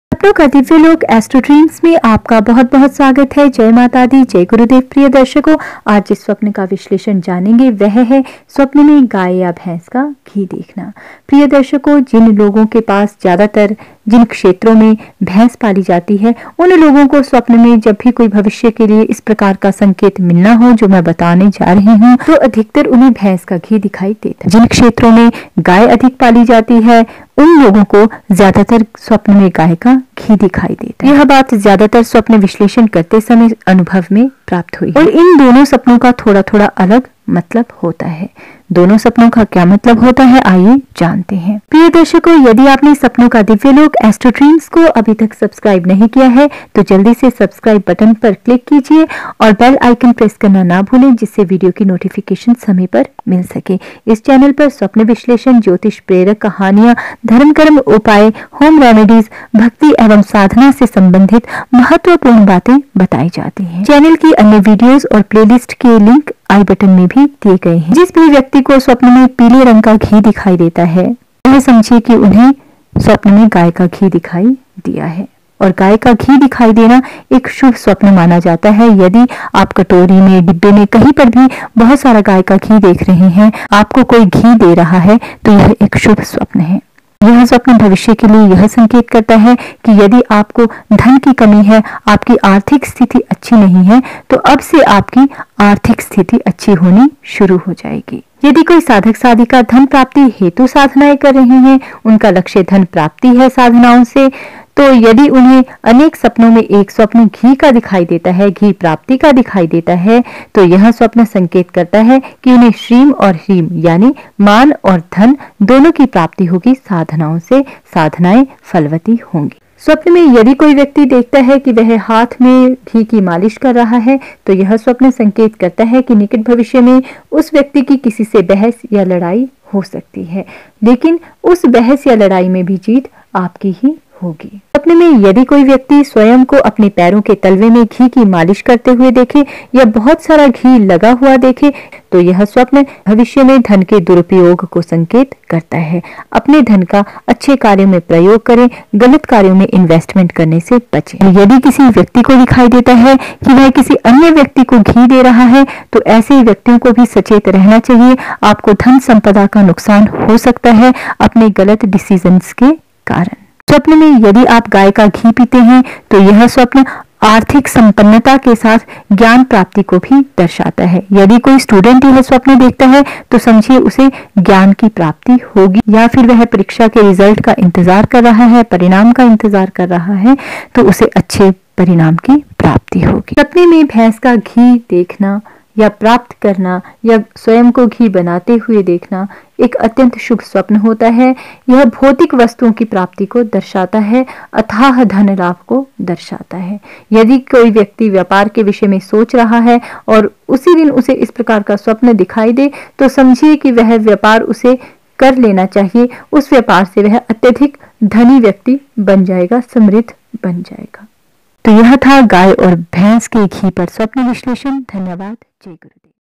सपने में घी देखना|Sapne me ghee dekhna
सपने में  गाय या भैंस का घी देखना सपने में घी हाथों में लगाना पांव में लगाना सपने में भैंस का घी देखना इत्यादि फलों का विश्लेषण जाने इस वीडियो के द्वारा
||Sapno ka Divyalok AstroDreams ll